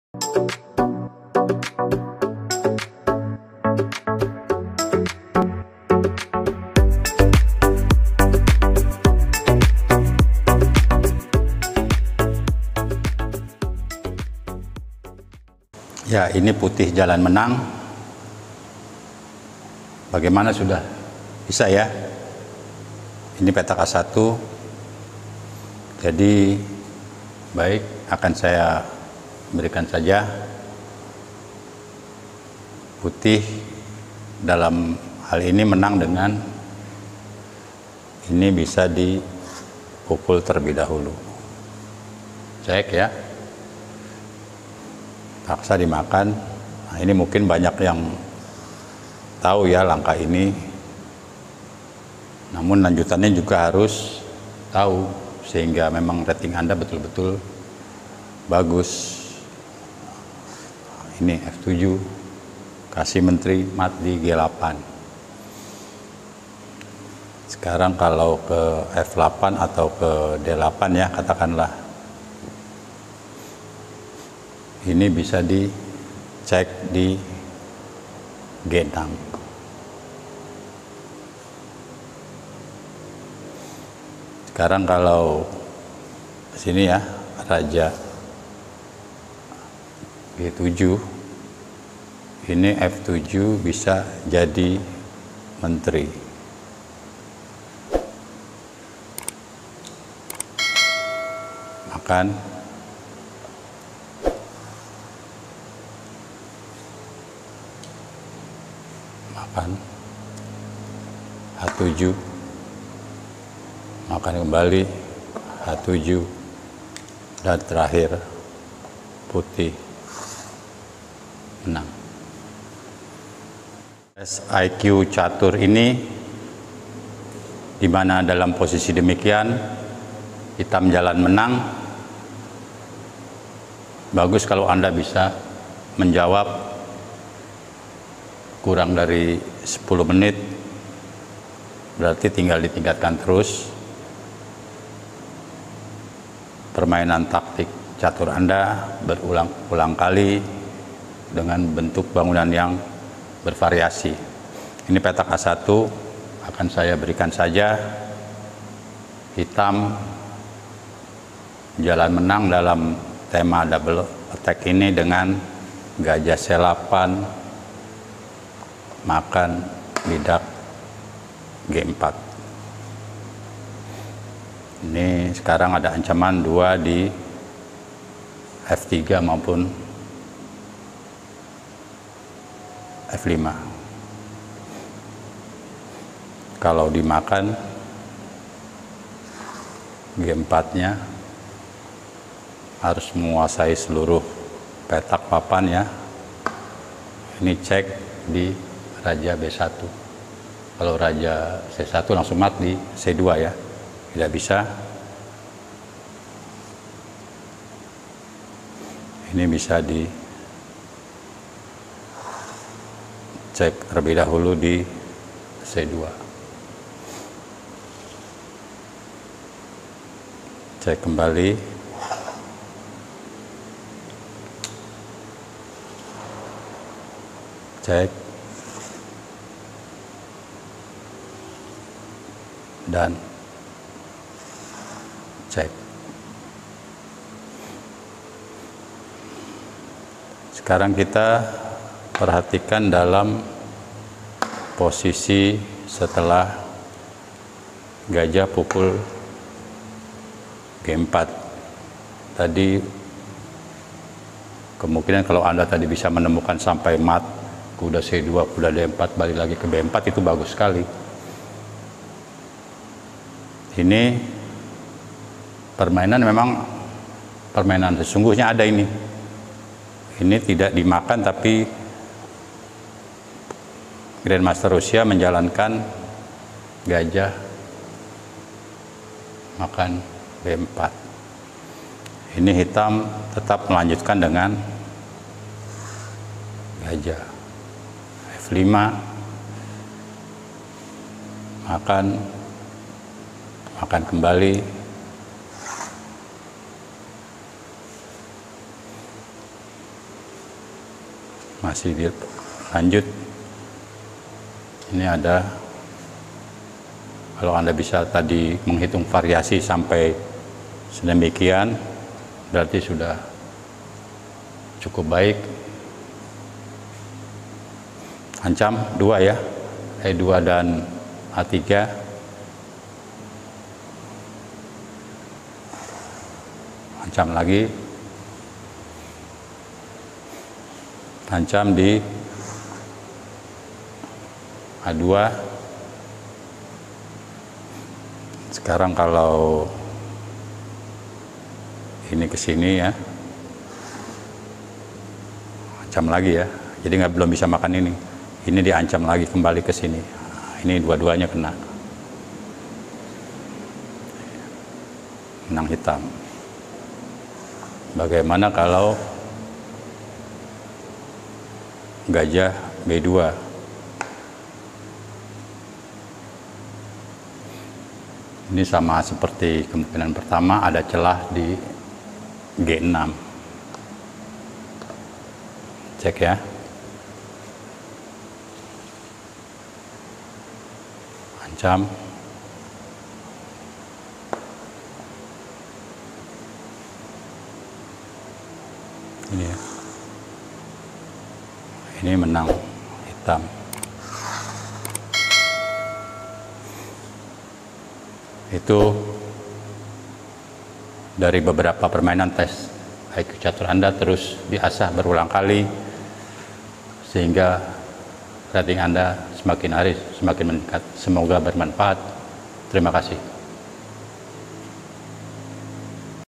SKILL bisa NAIK jika sering menjawab soal tes iq catur dari dunia catur rating 2200 tahun 2022
Hai semuanya ketemu lagi di Jago Catur, video kali ini akan diperlihatkan jawaban dari puzzle catur untuk rating catur 2200+ khususnya tema double attack atau tema serangan ganda yang sering muncul di pertandingan maupun permainan sehari-hari.
Video terdiri dari 2 studi catur yang diambil dari buku terkenal karangan pelatih catur terkemuka Rusia, August Livshitz.
Untuk video catur terbaik kali ini akan diperagakan soal tes IQ catur untuk rating kisaran 2200+.
Sistim catur dengan memecahkan tema catur seperti ini
pernah dipergunakan selama puluhan tahun kepada pecatur muda Rusia pada saat itu seperti mantan juara dunia Anatoly Karpov, Balashov, Sveshnikov dll.
Jago catur channel kedepannya juga akan menghadirkan video lainnya dari buku karangan Livshitz selain tema double attack, seperti discovered check, pin, decoy, x-ray dll.
Dengan berulangkali memecahkan tema-tema catur seperti ini dapat meningkatkan kemampuan taktik anda dalam bermain catur. Ayo tingkatkan terus kemampuan catur anda dengan mempelajari bentuk bentuk bangunan catur yang berkwalitas.

Tes IQ catur
video jago catur channel
dunia catur

Untuk anak-anak atau Orang Dewasa yang belum bisa main catur, banyak yang sudah bisa belajar disini ⬇️

[dari Chess24 premium]

Creator :
FM Harry Ubayana